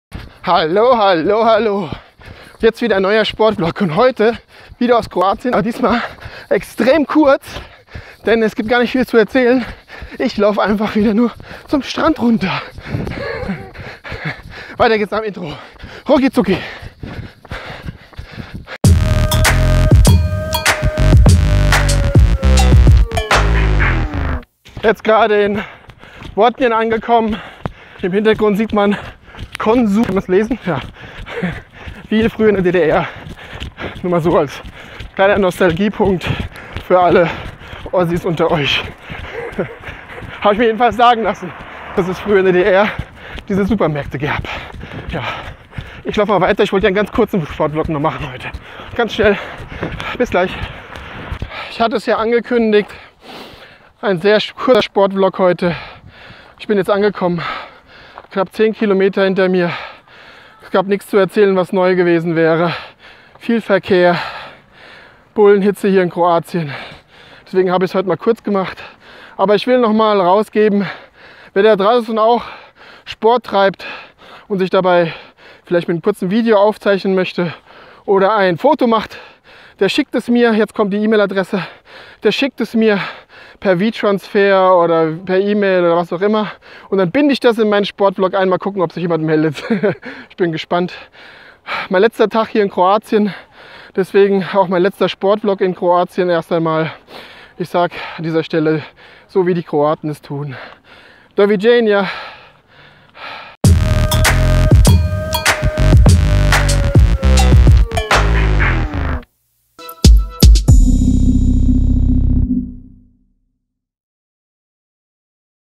Kurz und schmerzlos in Kroatien | Sportvlog#44 | Training Jogging Fitness
Einmal durchgelaufen zum Strand in Kroatien. Kurz und schmerzlos!